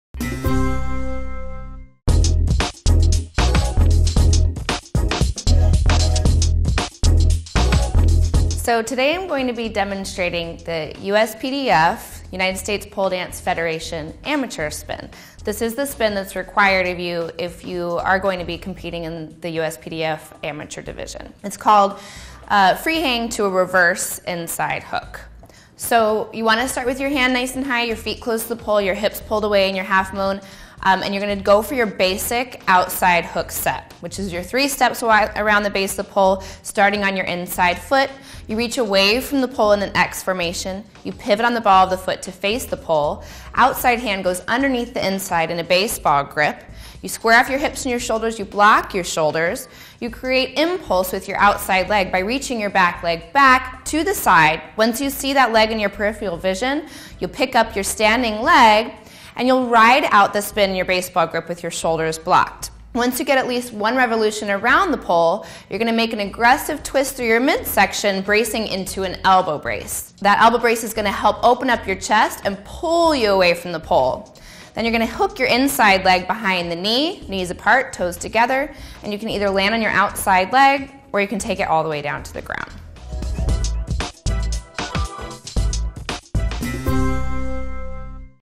USPDF Amateur Compulsory Spin | Pole Dancing
Today I am going to be demonstrating the USPDF, United State Pole Dance Federation, amateur spin.  This is the spin that's required of you if you're going to be competing in the USPDF amateur division.  It's called free hang to a reverse inside hook.  You want to start with your hand nice and high, your feet close to the pole, your hips pulled away in your half moon and you're going to go for your basic outside hook step which is you're three steps wide around the base of the pole starting on your inside foot.  You reach away from the pole in the x formation, you pivot on the ball of the foot to face the pole, outside hand goes underneath the inside in the baseball grip.  You square off your hips and your shoulders. You block your shoulders.  You create impulse with your outside leg by reaching your back leg back to the side.  Once you see that leg in your peripheral vision you'll pick up your standing leg and you'll ride out the spin - your baseball grip with your shoulders blocked.  Once again, at least one revolution around the pole, you're going to make an aggressive twist to your midsection bracing into an elbow brace.  That elbow brace is going to help open up your chest and pull you away from the pole.  Then you're going to hook your inside leg behind the knee, knees apart, toes together.  You can either land on your outside leg or you can take it all the way down to the ground.